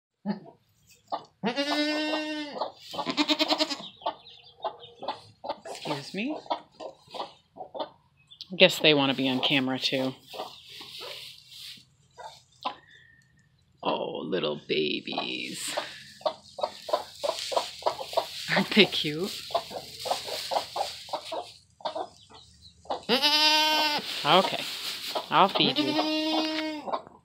Snuggle Bunny the Hen with her Babies (and a goat photo bomb)
We let our hens hatch their own chicks in the spring of 2017 and it was so much fun to watch them grow!